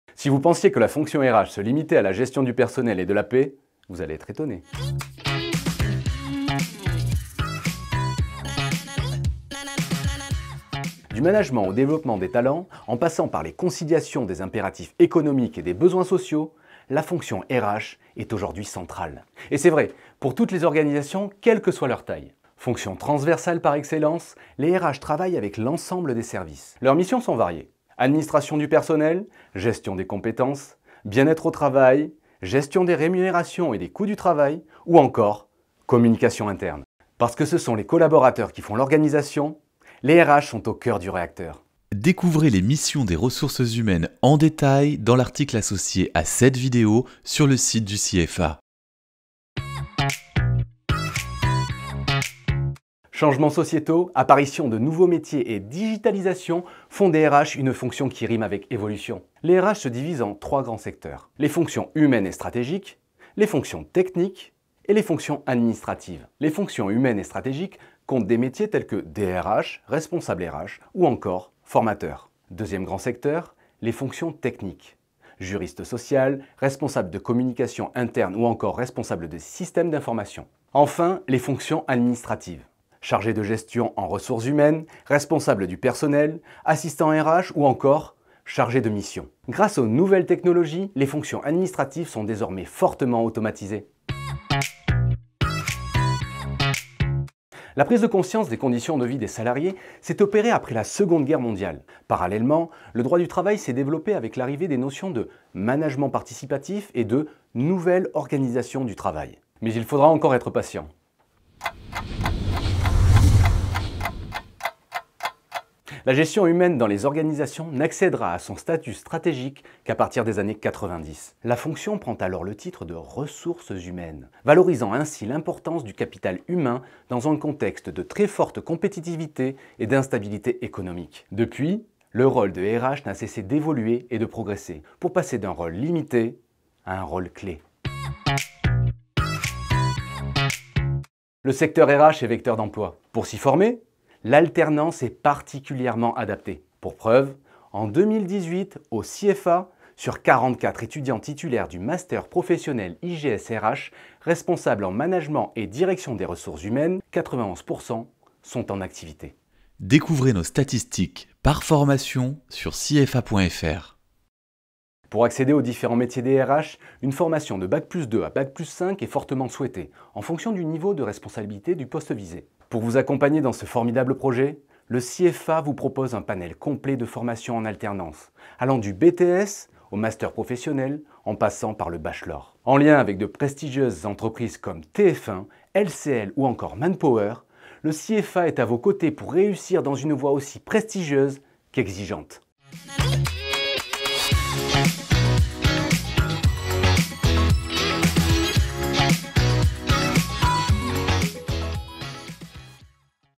FILIÈRE RH | Comment travailler dans les Ressources Humaines ?
Les Ressources Humaines jouent désormais un rôle clé dans l'organisation des entreprises. Mais comment y travailler ? Quelles études faire ? Quels en sont les débouchés ?